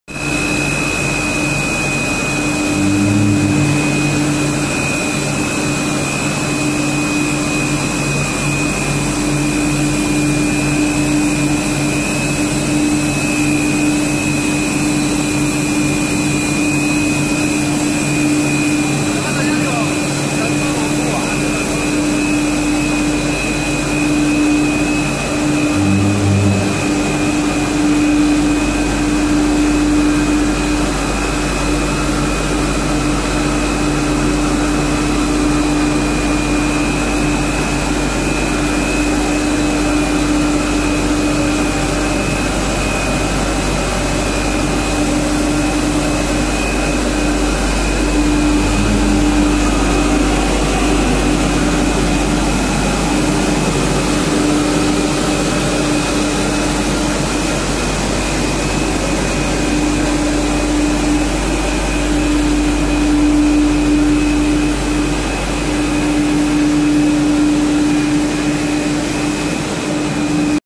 HDPE Double Wall Corrugated Pipe Extrusion Line
Raw Material: HDPE material
Designed Machnical Speed: 1-3.8 m/min
Mould Blocks cooling: Water ( Vacuum system)
Control System: Siemens